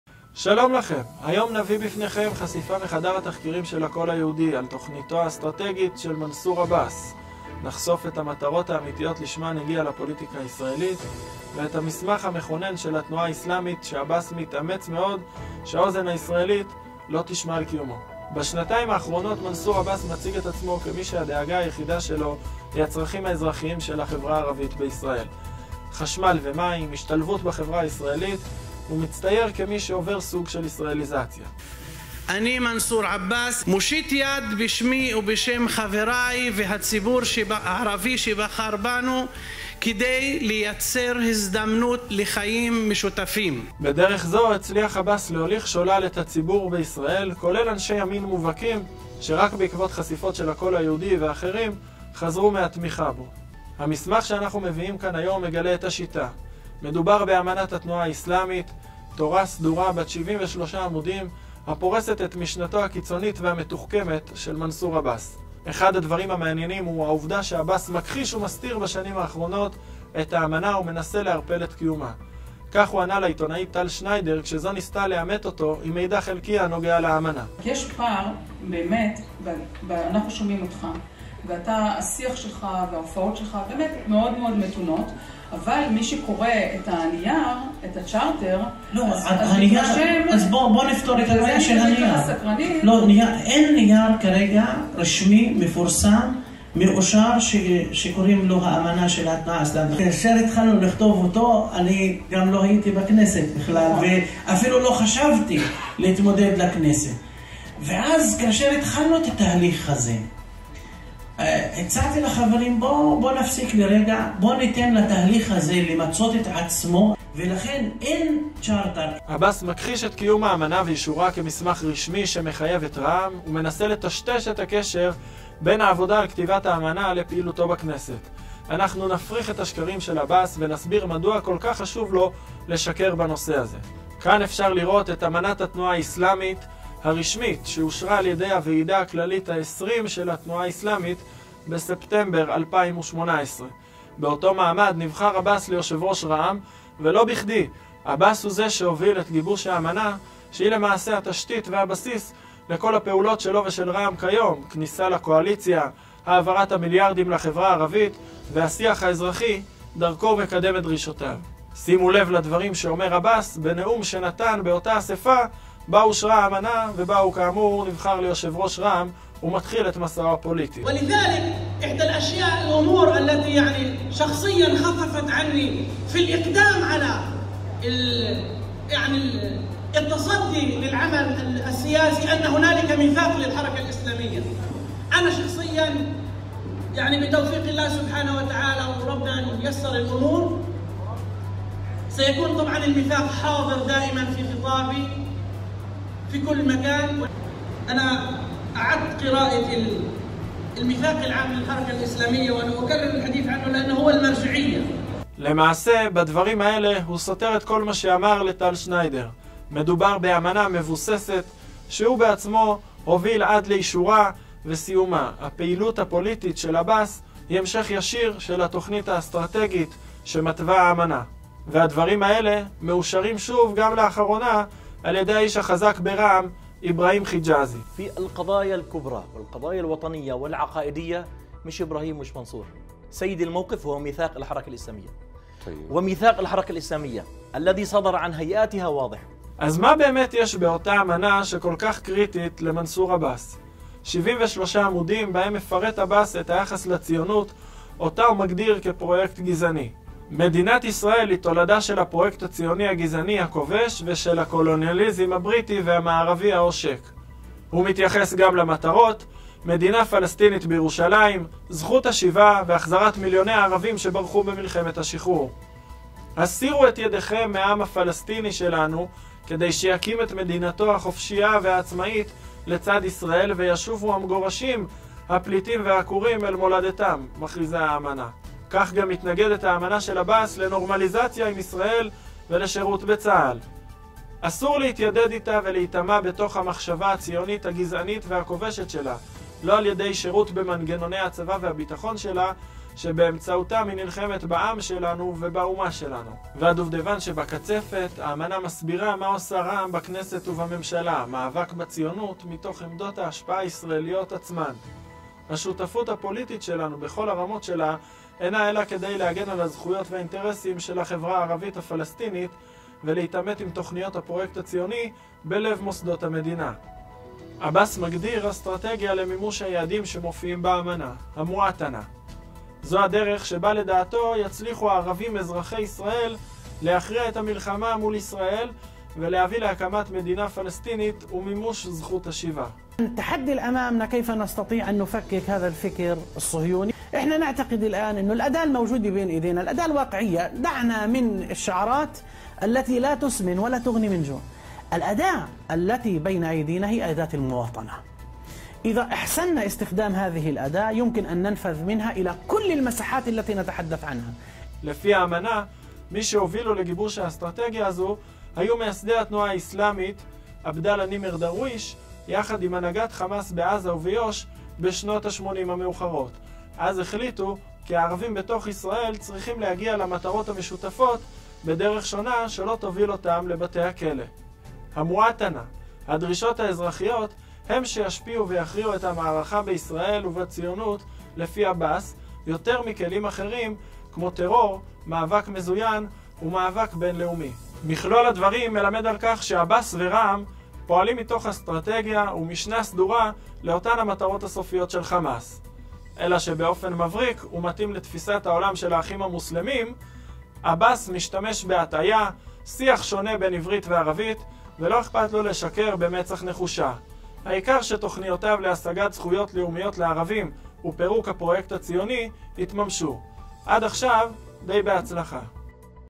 תחקיר הקול היהודי: האסטרטגיה של מנסור עבאס ואמנת רע"מ נחשפת
קטעים שאנו מפרסמים לראשונה מאמנת התנועה האסלאמית חושפים את התכנית: פעילות פוליטית כדי להיאבק בציונות מלב מוסדותיה, להביא לזכות השיבה וזכויות לאומיות לערבים בישראל. התחקיר מוכיח כי בניגוד לדברי עבאס הוא זה שהוביל את האמנה ואישורה כמסמך מחייב - צפו

פנינו למפלגת רע"מ, שם לא מסרו תגובה לדברים.

תחקיר: אלחנן גרונר ומאיר אטינגר
תרגום מערבית: מורן טל
עריכה: אביה נתן.